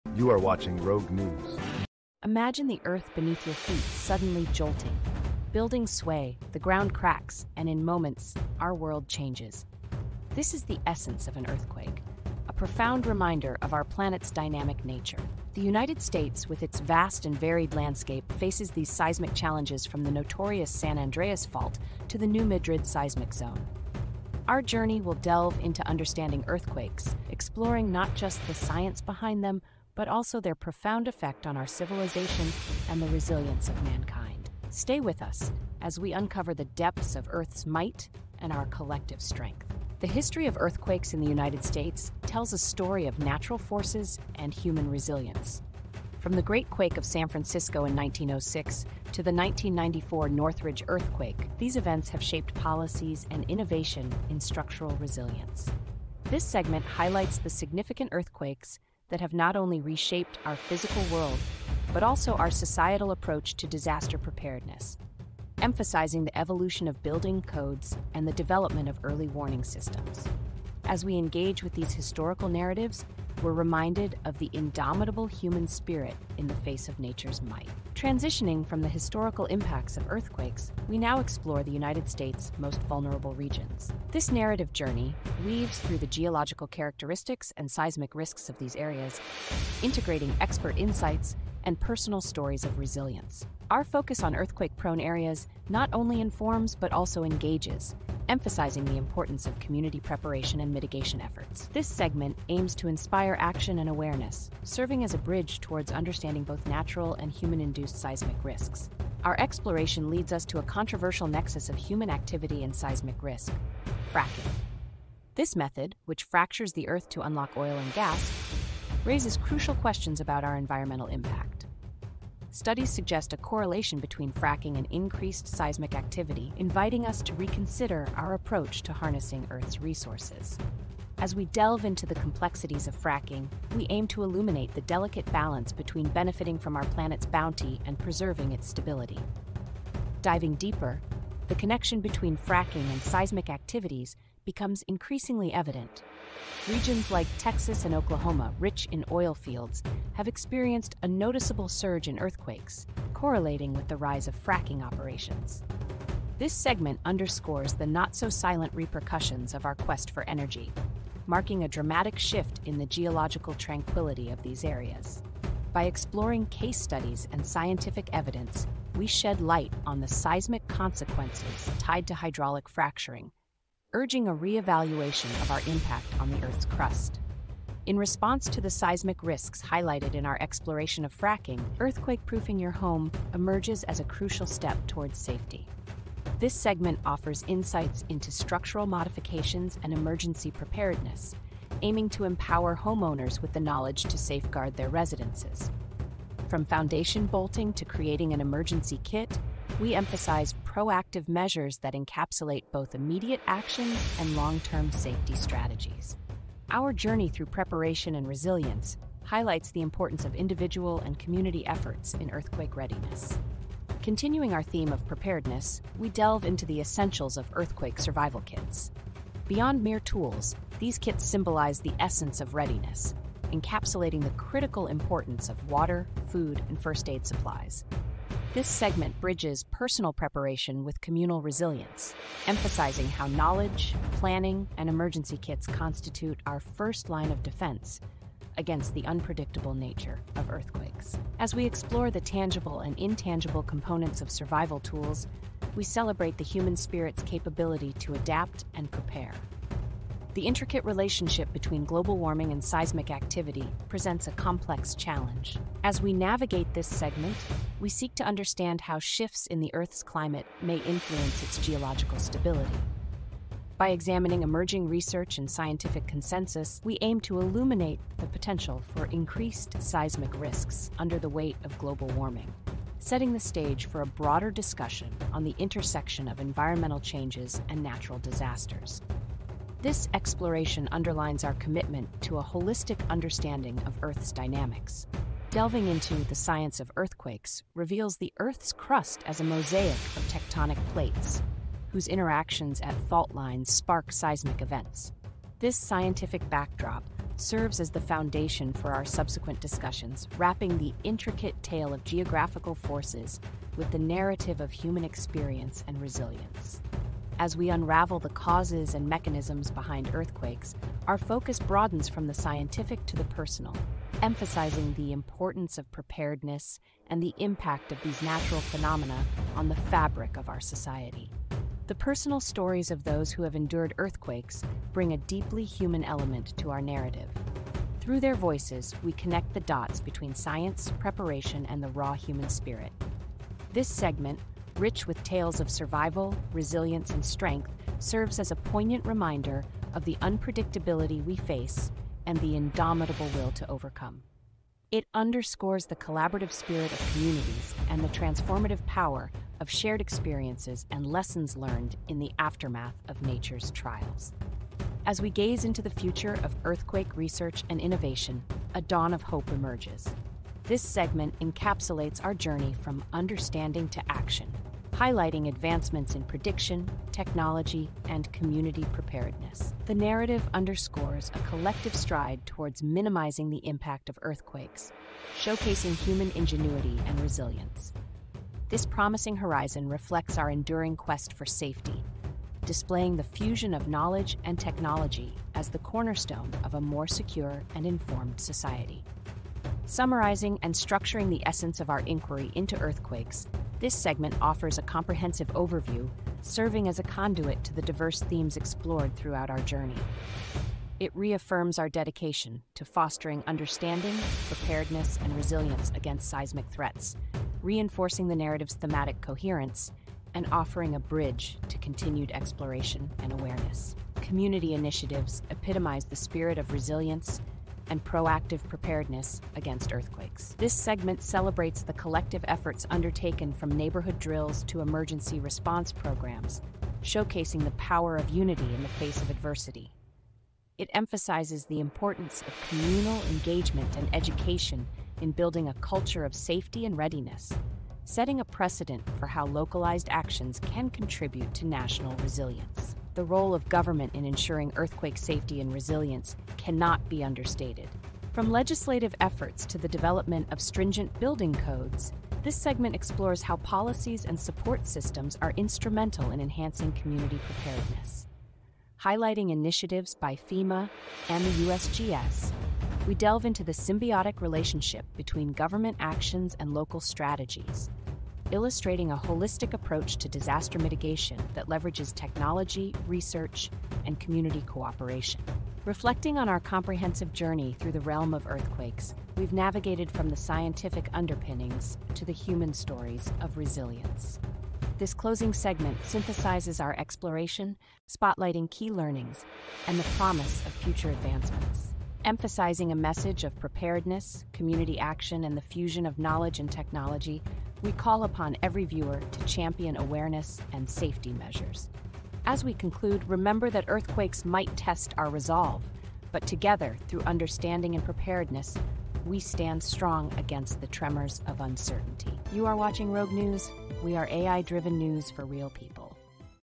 Shaken Ground: Unveiling Earthquake Realities in the U S
Dive into the seismic world of earthquakes in the United States with our comprehensive documentary. Explore the causes behind these natural disasters, pinpoint their most common locations, and learn about crucial safety measures to protect yourself. From the impact of fracking to the potential effects of global warming on seismic activity, gain insightful knowledge on how to stay safe in an ever-changing environment.